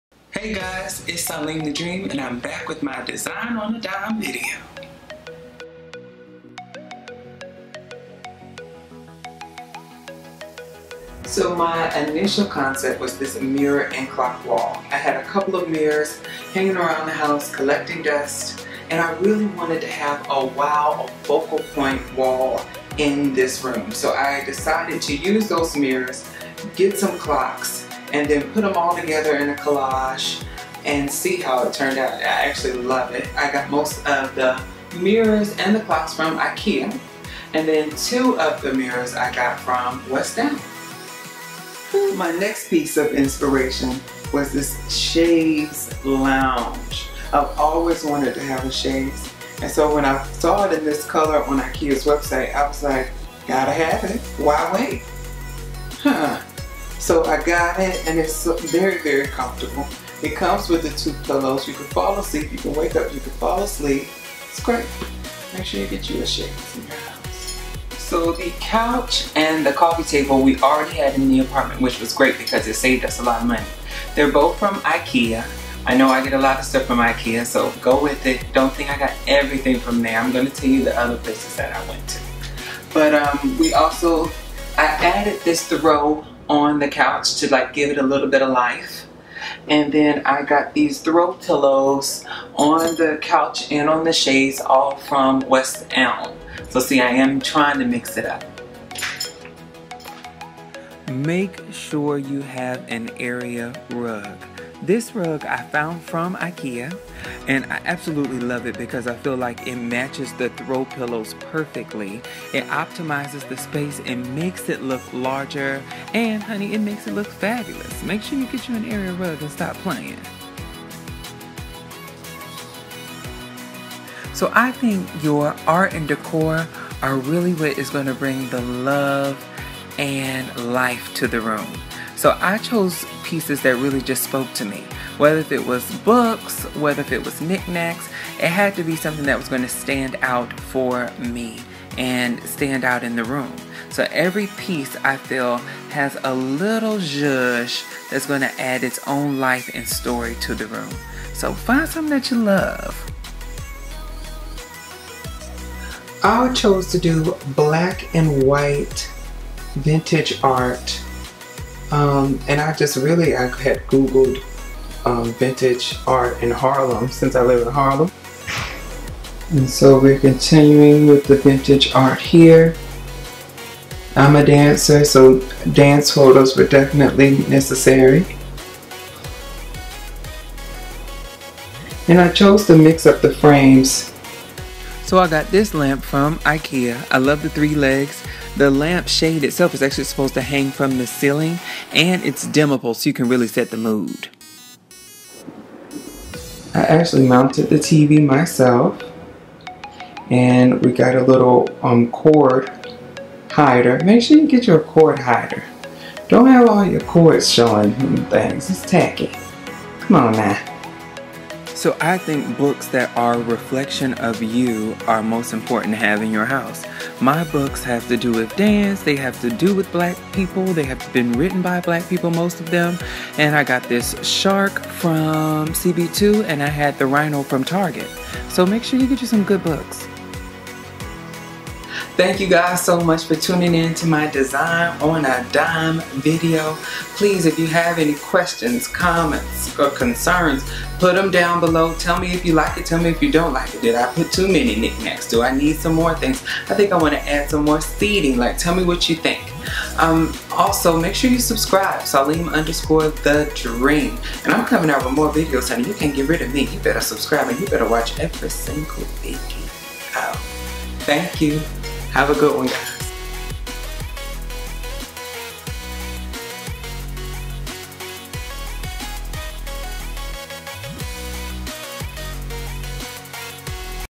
How To: Design on a Dime with Saleem_thedream
How to Interior Design. Interior Design Tips For Every Budget from Saleem_thedream.

Ikea:
Chaise
Mirrors
Clocks
Couch
Coffee Table
Curtains
Curtain rods
Under TV Shelves
Dark Brown Lamp

West Elm:
Mirrors
Pillows
Gold Frog

CB2:
Faux Plant
Glass Balls and Dish
Wired Taxidermy
Shark
5 Minute Hour Glass
Dinosaur
Vases

Target:
Silver Lamp
Rhino
1 Vase
Lotus Flower

Urban Outfitters:
Curtain Hooks
2 Books

Books

TV

Jet.com
Nelson Mandela Bust

Home Depot:
Portable A/C